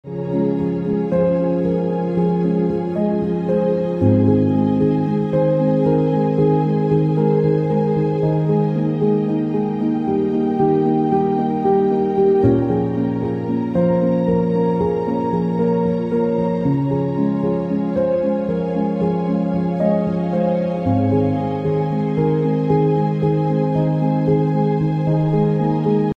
画本霞浦 这里是福建霞浦，一个拥有美丽滩涂的闽东小城，无论日出时分还是日落时分，这里的霞光总是美不胜收。
因为你可以通过云层的变化、海面的起伏，感受到整个空间光影的瞬息万变。就像一幅山水画里的笔墨节奏，大海为纸，光影为笔，山川之间，想象力便可以绘出诗情万里。